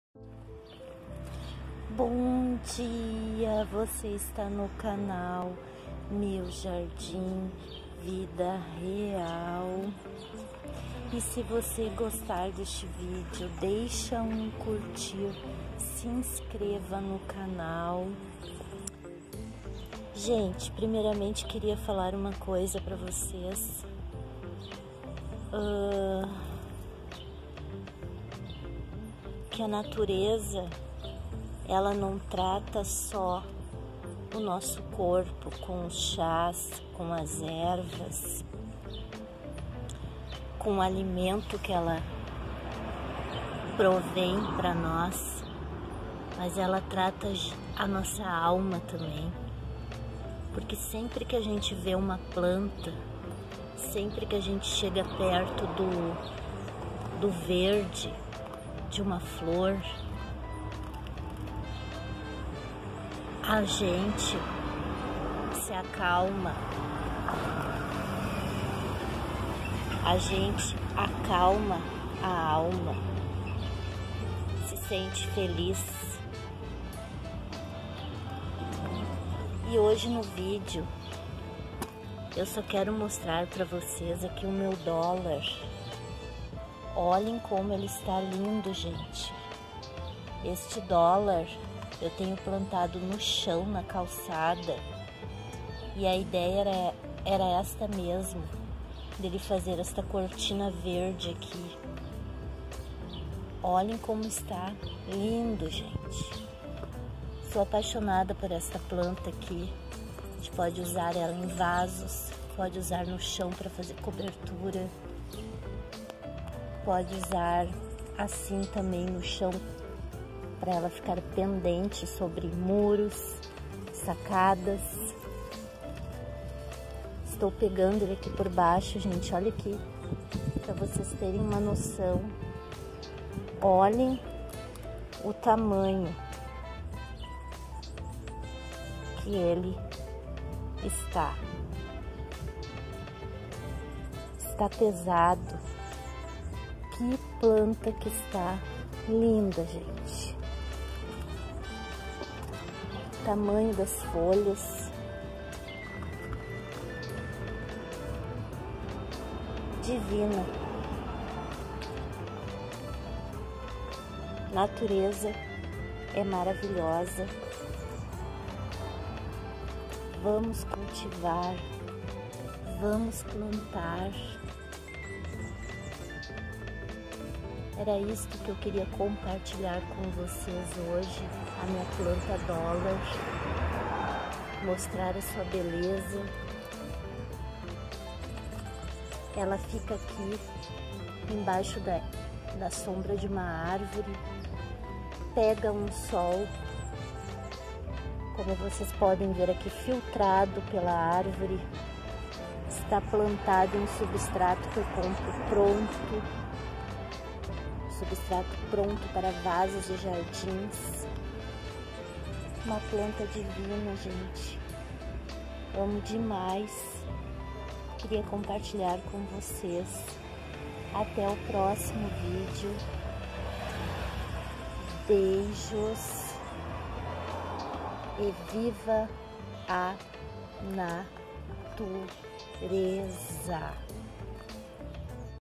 Planta Dólar, plantada no chão do jardim, formando uma cortina verde linda 😍
Planta Dólar, uma planta de vó e de fácil cultivo, que traz muito verde ao jardim!